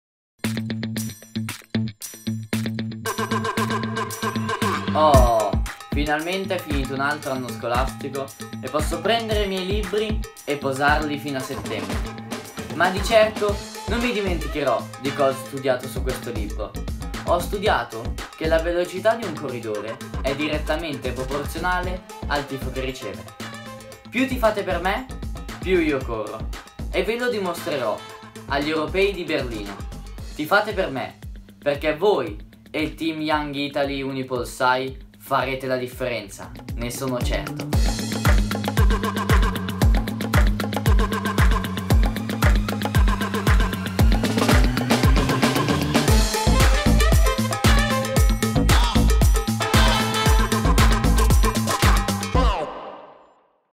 TeamYoungItalyUnipolSai | Support Riccardo Il Proiettile Azzurro
La velocità di un corridore è direttamente proporzionale al tifo che riceve.
Noi siamo già ai blocchi di partenza pronti a tifare per te Riccardo Bagaini agli europei di berlin2018.